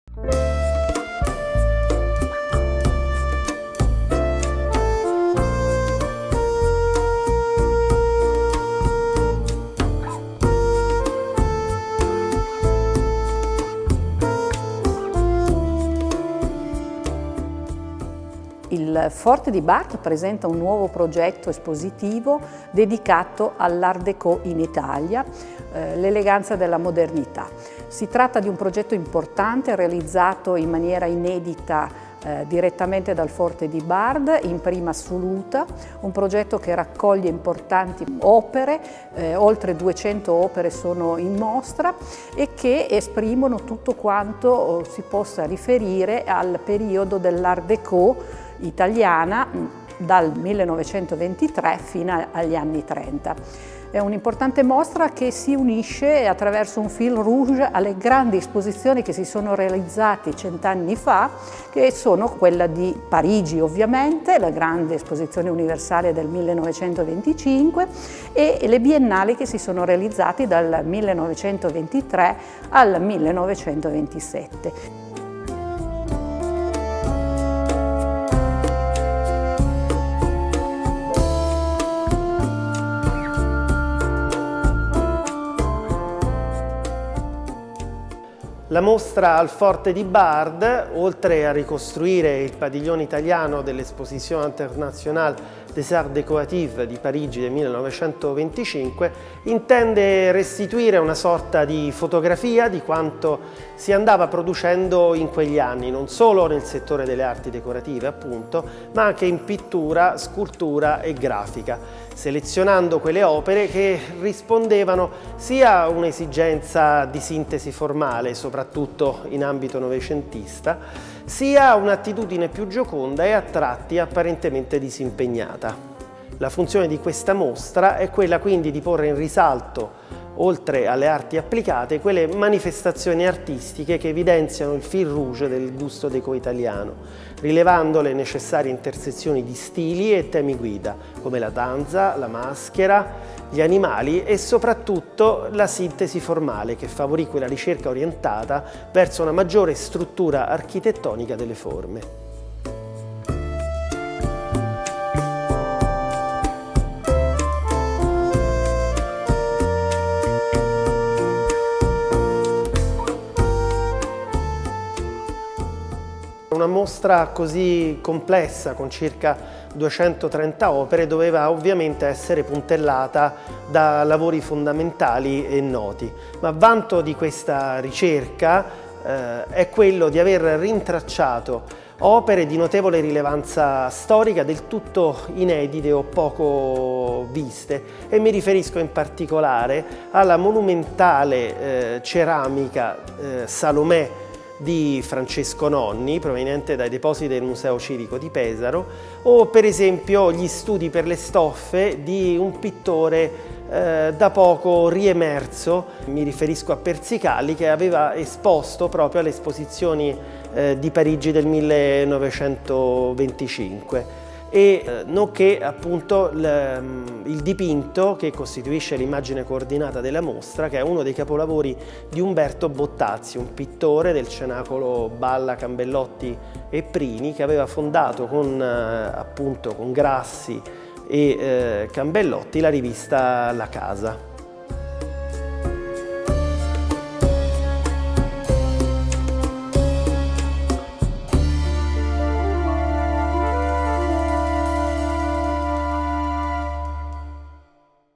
Il Déco in Italia. L'Art Déco al Forte di Bard
Il progetto espositivo promosso da Forte di Bard e Silvana Editoriale presentato dal curatore della mostra Francesco Parisi e dalla presidente dell'Associazione Forte di Bard, Ornella Badery.

Sino al 10 aprile 2023. Forte di Bard. Valle d'Aosta.